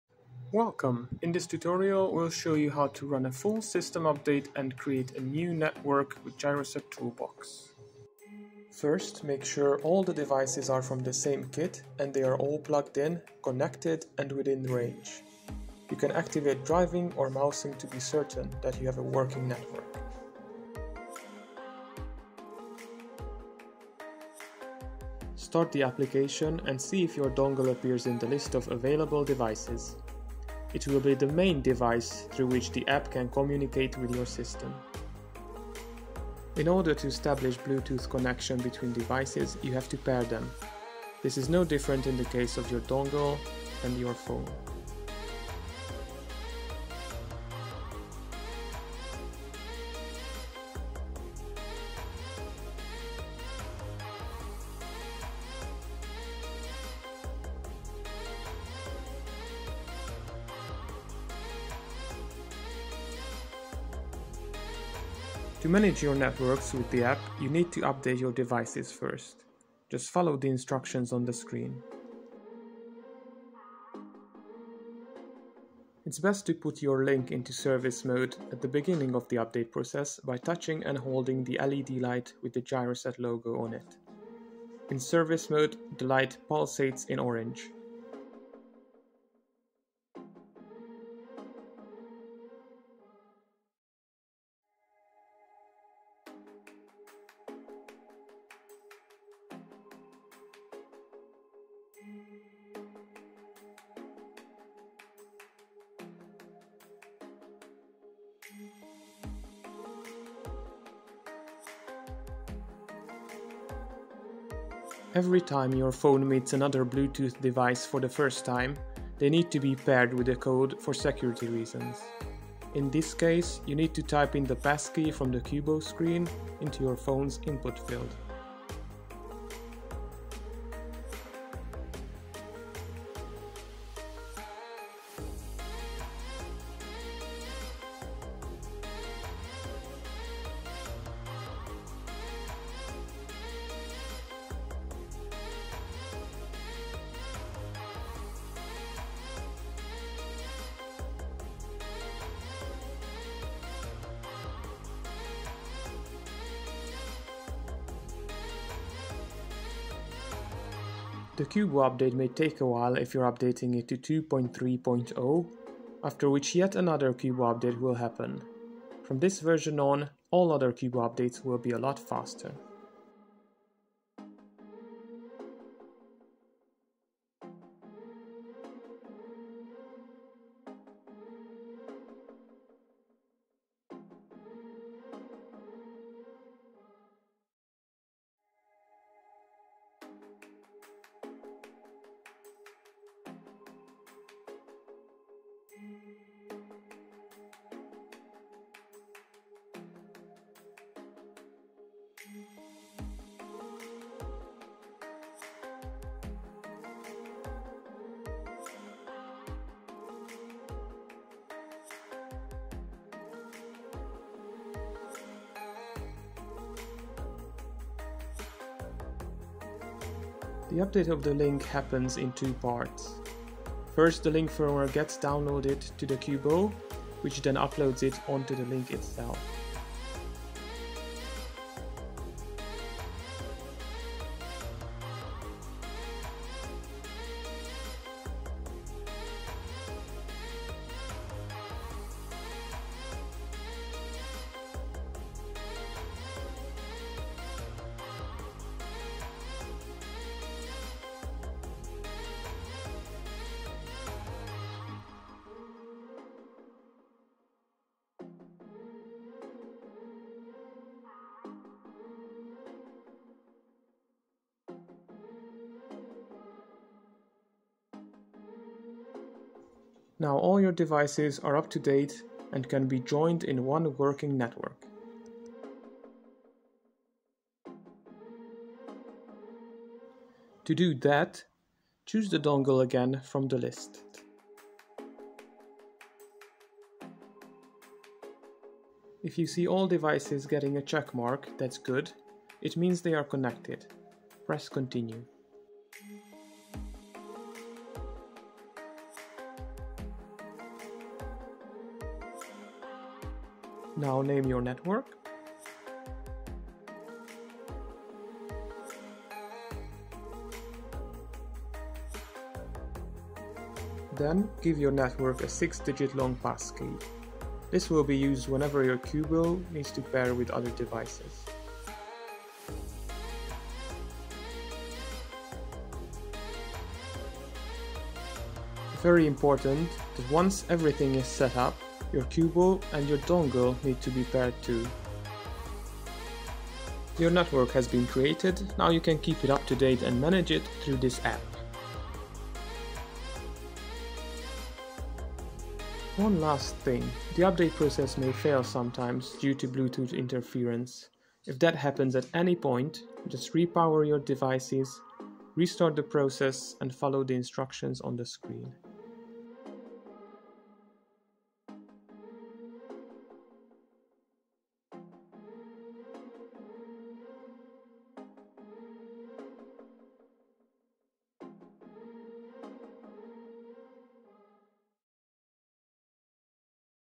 GyroSet™ Toolbox - How to update your system and create your first network
In this tutorial video we present how to update a full GyroSet™ Head Drive system and how to create your first network with our service app, GyroSet™ Toolbox.

The application is available for free on Google Play Store and on the App Store.